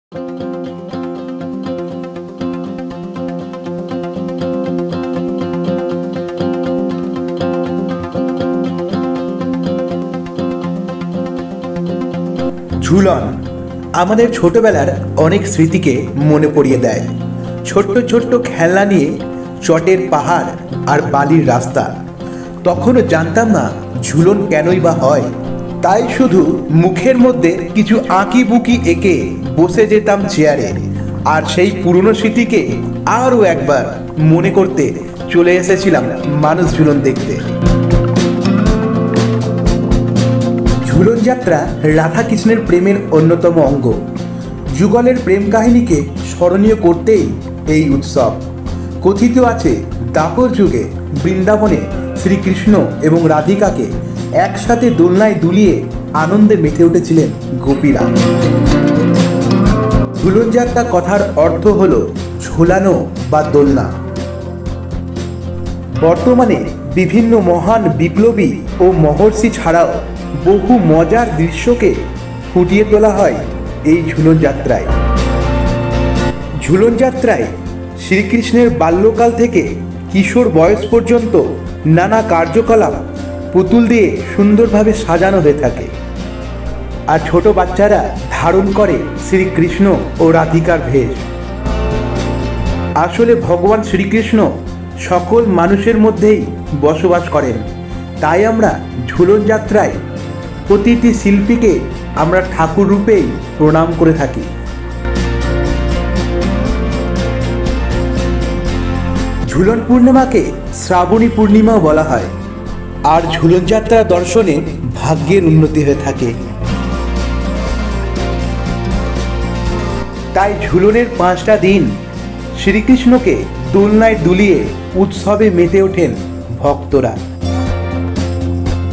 পাথরে তৈরি মানুষ । না মানুষ ঝুলন। অবাক করা এক দৃশ্য।
বন্ধুরা এবারে আমরা পৌঁছে গেছিলাম ঝুলন যাত্রা দেখতে তারই কিছু দৃশ্য আপনাদের সঙ্গে তুলে ধরলাম।

this video is just for  entertainment only. awaz channel does not claim this to talk this stories nor does it promote any kind of superstition